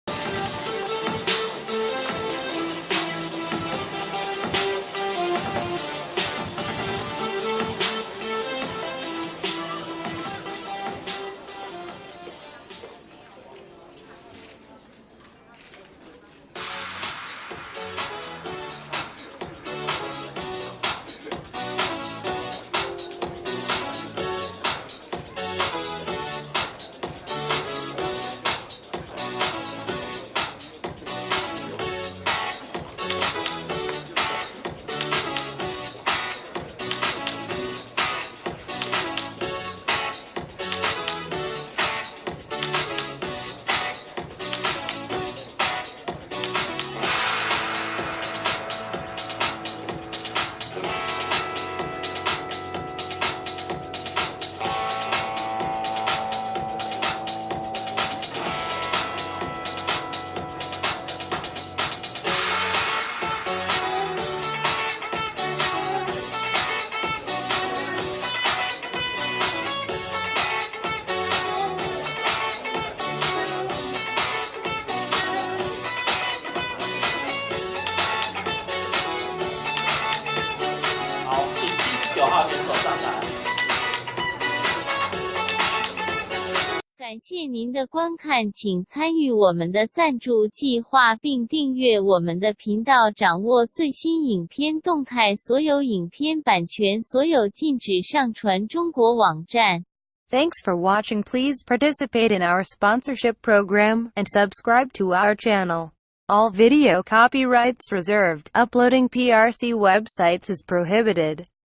【無限HD】108年嘉義市市長盃全國健美錦標賽 女子比基尼 163CM以上 自選動作 78號(4K HDR)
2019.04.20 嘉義